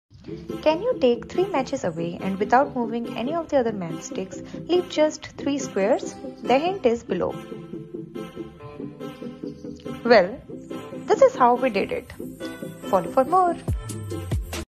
Matchstick Puzzles
Do you love puzzles?🧩
Solving puzzles has a significant impact on a child's development, such as attention span, cognitive development, early maths, eye-hand coordination, fine motor ability, language, memory, perseverance, problem-solving skills, relaxation, confidence, topic-specific knowledge, and visual perception!

Try out this fun puzzle with your kids!.